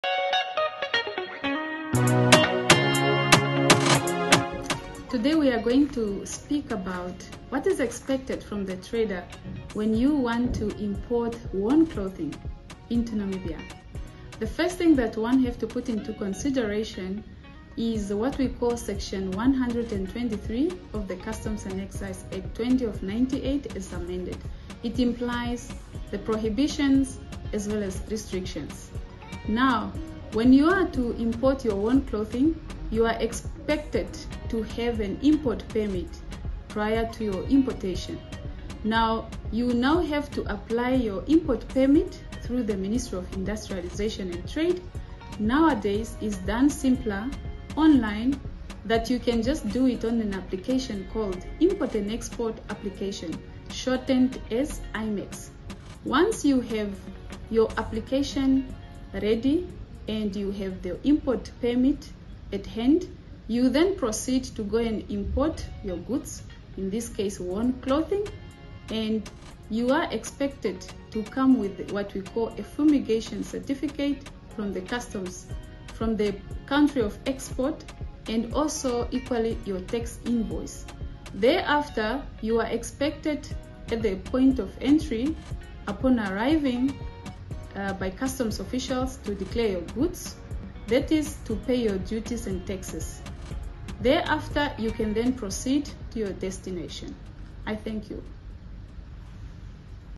Requirements - Importing second-hand clothing
If you're considering importing second-hand garments and giving them a new lease on life, here's the scoop on our importing requirements for second-hand clothing.

Here's what you need to know: